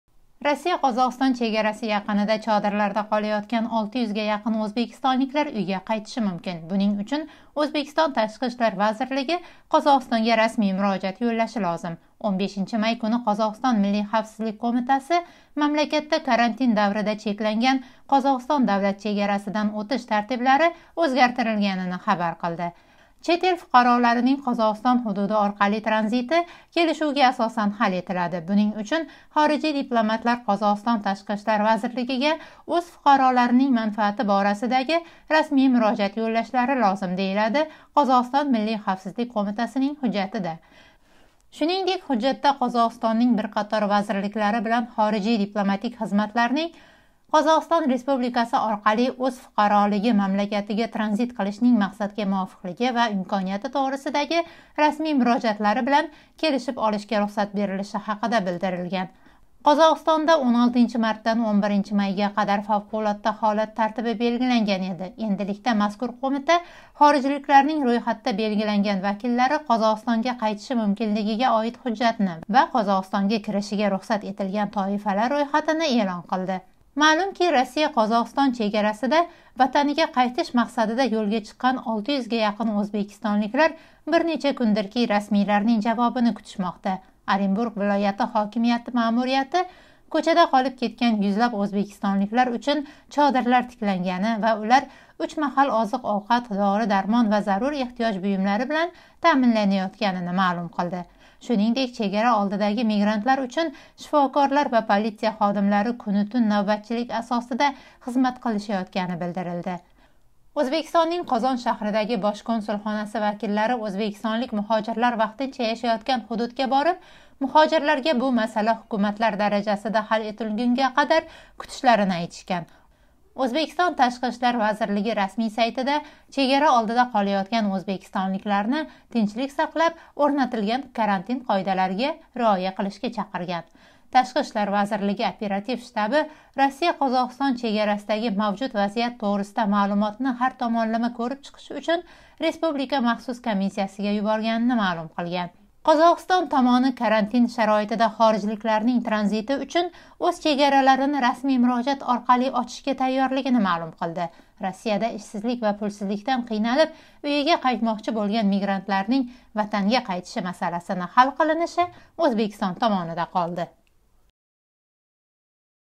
Қозоғистон мигрантларга чегарани очади, бироқ шарти бор - Ўзбекистон
Россия-Қозоғистон чегараси яқинида чодирларда қолаётган 600 га яқин ўзбекистонликлар уйга қайтиши мумкин. Бунинг учун Ўзбекистон Ташқи ишлар вазирлиги Қозоғистонга расмий мурожаат йўллаши лозим.

15 май куни Қозоғистон Миллий хавфсизлик қўмитаси мамлакатда карантин даврида чекланган Қозоғистон давлат чегарасидан ўтиш тартиблари ўзгартирилганини хабар қилди.